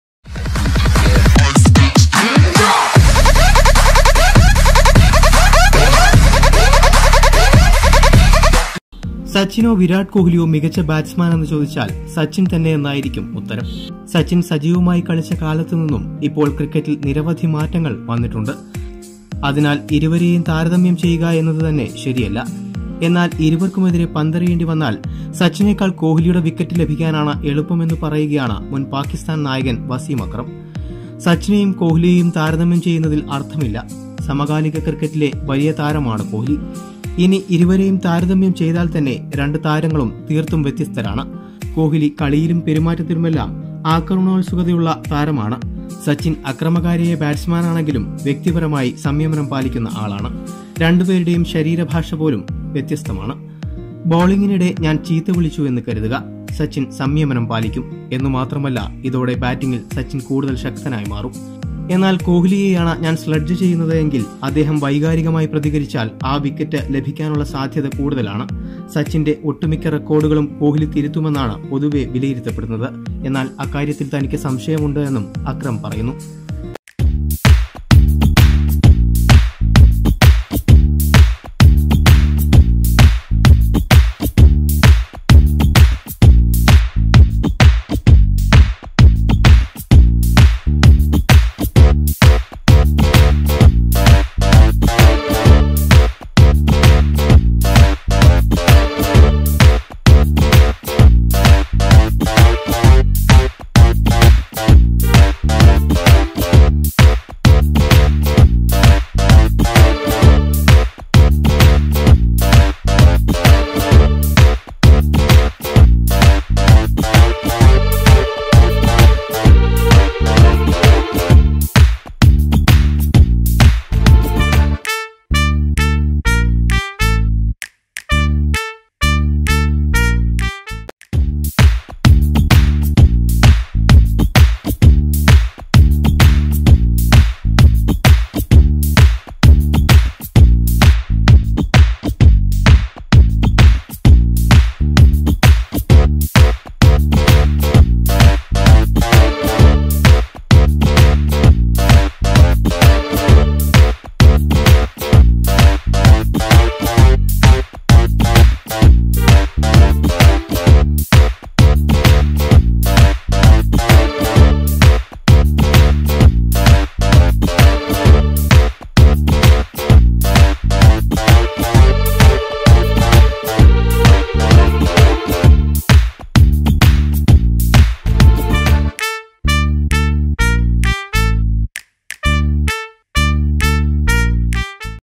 രണ്ട് താരങ്ങളും തീർത്തും വ്യത്യസ്തരാണ്
സച്ചിൻ സച്ചിനോ വിരാട് കോഹ്‌ലിയോ മികച്ച ബാറ്റ്സ്മാൻ എന്ന് ചോദിച്ചാൽ സച്ചിൻ എന്നു തന്നെയായിരിയ്ക്കും ഉത്തരം. സച്ചിൻ സജീവമായി കളിച്ച കാലത്തുനിന്നും ഇപ്പോൽ ക്രിക്കറ്റിൽ നിരവധി മാറ്റങ്ങൾ വന്നിട്ടുണ്ട്. അതിനാൽ ഇരുഇവരെയും താരതമ്യം ചെയ്യുക എന്നത് തന്നെ ശരിയല്ല. എന്നാൽ ഇരുവർക്കുമെതിരെ പങ്കെറുയേണ്ടിവന്നാൽ, സച്ചിനേക്കാൾ കോഹ്‌ലിയുടെ വിക്കറ്റ് ലഭിയ്ക്കാനാണ് എളുപ്പം എന്ന് പറയുകയാണ് മുൻ പാകിസ്ഥാൻ നായകൻ വസീം അക്രം.